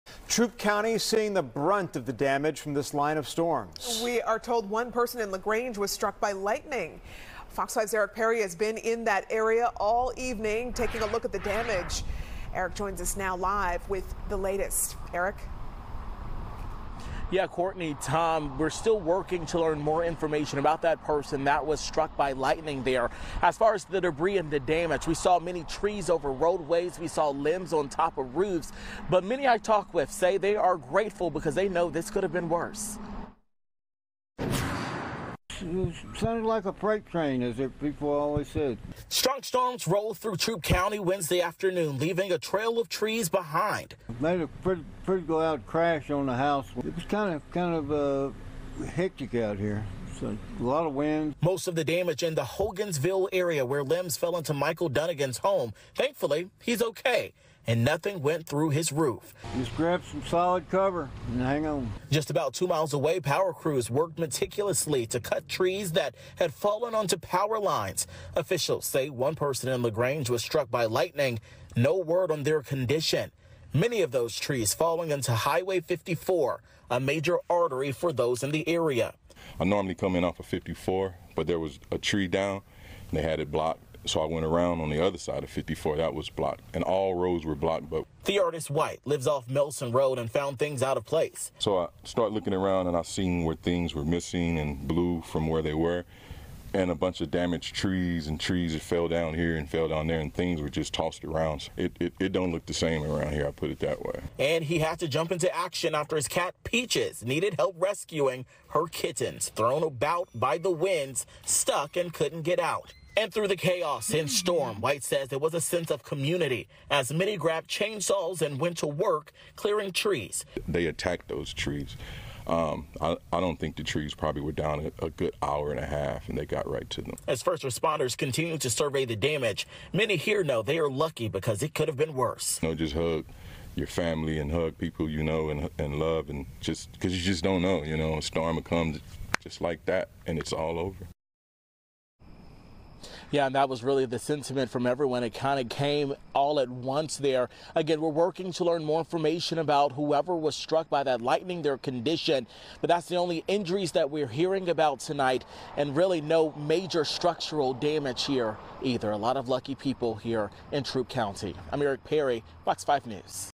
Man struck by lightning in strong Georgia storms | FOX 5 News
Troup County received the brunt of the damage from a line of storms Wednesday. There are reports of someone struck by lightning. Here's a look at the damage around the area and what officials are doing to clean it up.